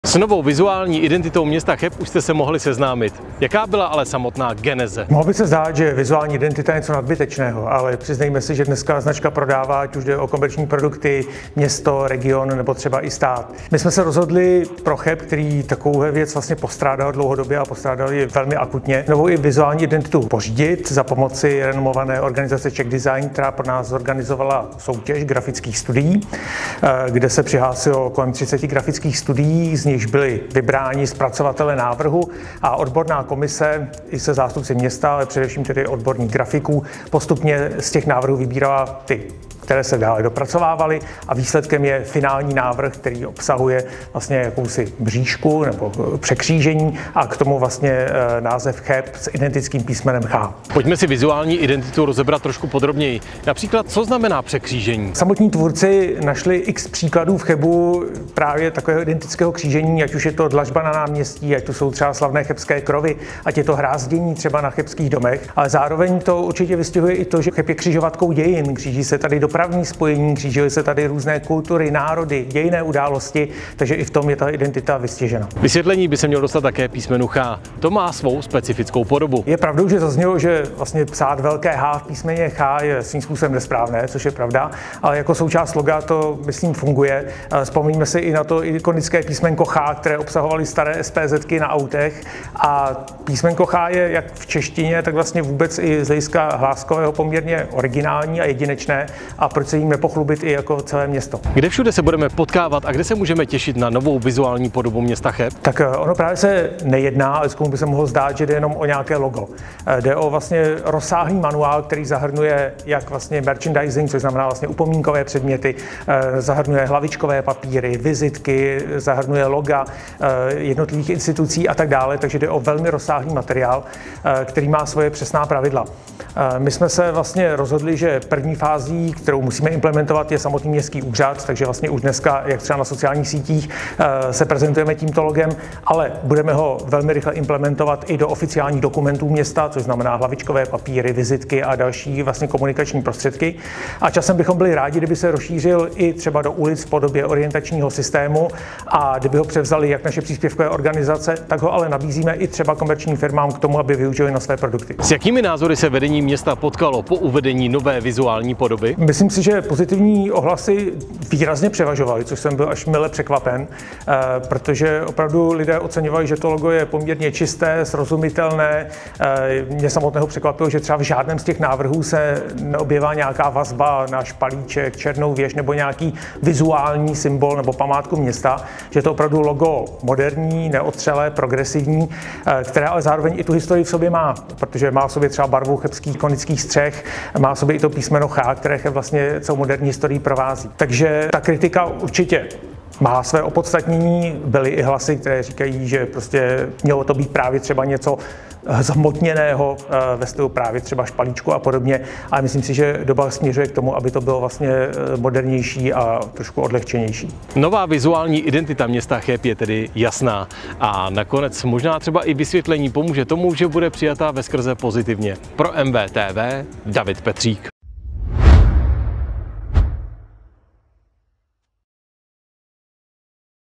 CHEB MÁ NOVOU MODERNÍ VIZUÁLNÍ IDENTITU
Vedení města Cheb vnímalo pořízení nové vizuální identity jako velmi potřebné. Městu pomohla soutěž vyhlášená agenturou Czechdesign. Starosta Antonín Jalovec k projektu říká: „Od začátku soutěže jsem akcentoval, aby byl výsledný návrh jednoduše komunikovatelný a jeho podoba snadno zdůvodnitelná obyvatelům i návštěvníkům našeho města. Jsem rád, že vizuál, který ze soutěže vzešel, toto jednoznačně splňuje. Získáváme nástroj, který jistě dokáže Chebany oslovit a který nám pomůže prezentovat Cheb jako moderní a atraktivní město. Vidím v něm velký potenciál přispět k významnější identifikaci obyvatel s jejich městem, což bylo hlavním cílem celé akce. Jsme ale teprve v polovině práce. Máme v rukou prototyp, který nyní musíme uvést ‚do výroby‘, tedy implementovat do běžného života Chebu. Věřím, že se to podaří. Považuji to za osobní prioritu.“ V samotném vizuální ztvárnění dominují dva prvky překřížení a písmeno CH. Symbol mřížky je pro město autentický, najdeme ho na každém kroku v dláždění chebských ulic, v konstrukcích jedinečných historických krovů nebo třeba v městském znaku.